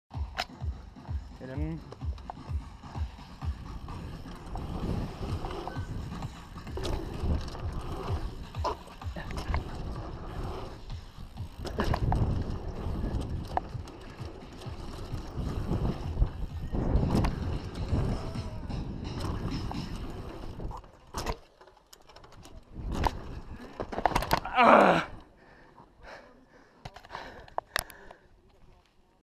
Matteo Pohořelice...
...progresivní jízda ze skoků v Pohořelicích od Matjůa